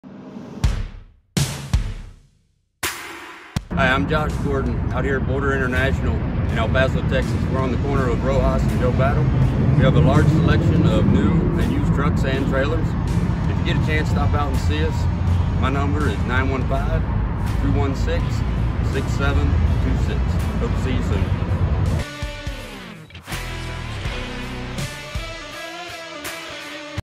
Josh Gordon | Truck and Trailer Sales
Josh has gained ground up knowledge and skills from his days as a technician, service advisor and service manager.